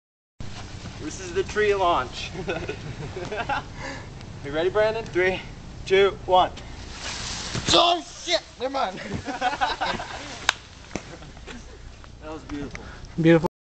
the tree launch
B-Random launching from a tree